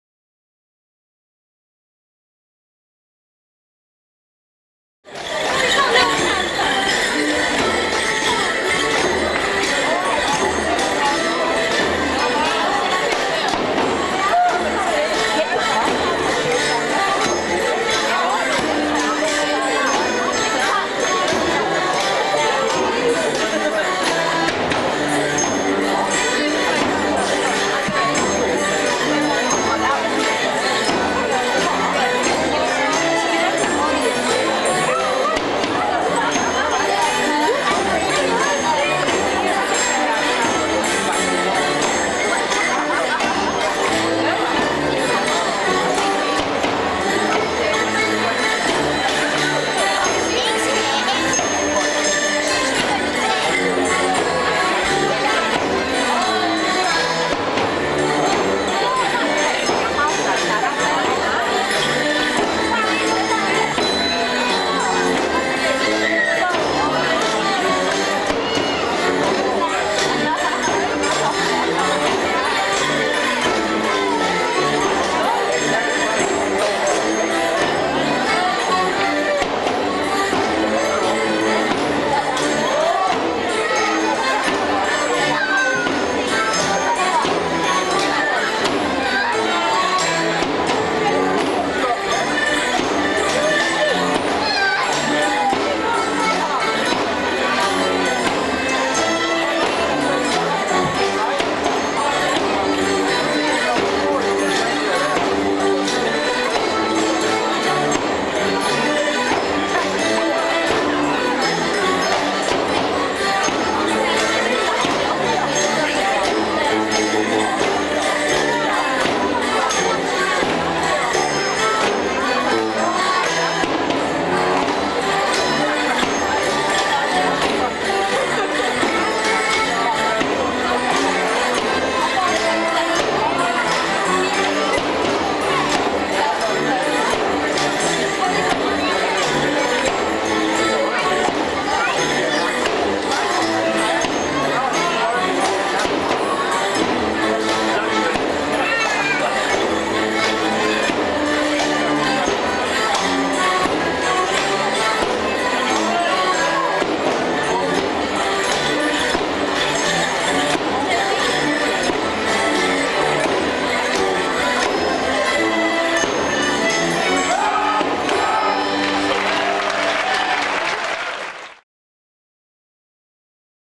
Silurian Border Morrismen - Upton on Severn Hankerchief Dance, Daughter of Dancing England 1993
Silurian performing The Upton Hanky Dance at "Daughter of Dancing England" in Derby Assembly Rooms in 1993